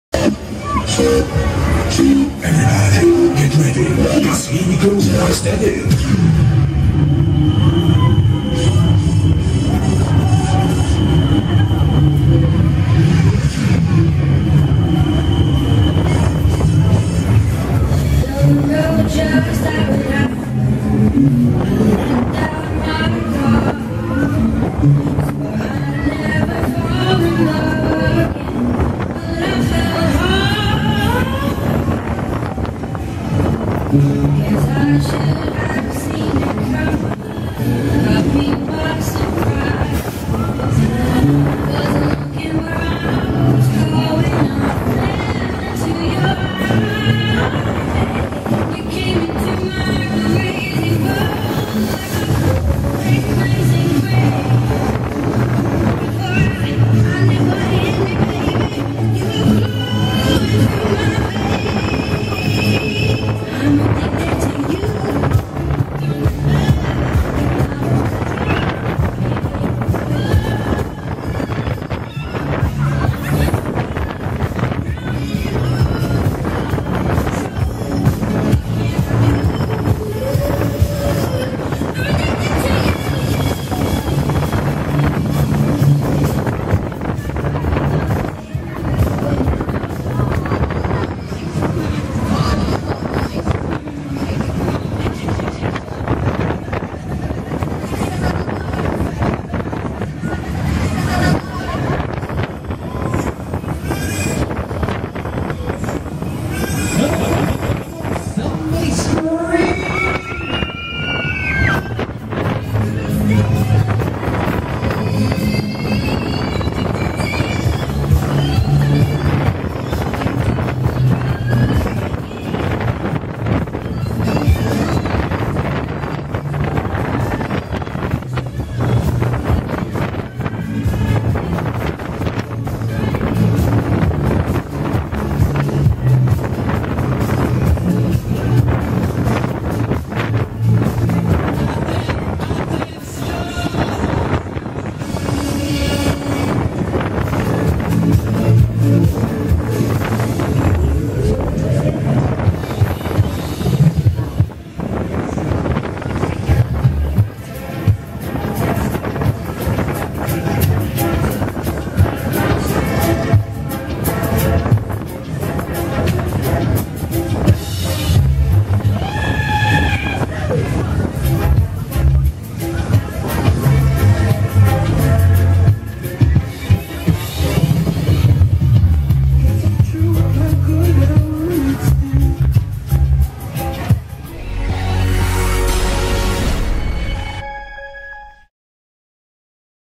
Leigh Danter's Superstar Onride @ Great Dorset Steam Fair 2014
Onride footage of Leigh Danter's Superides Superstar at Great Dorset Steam Fair 2014.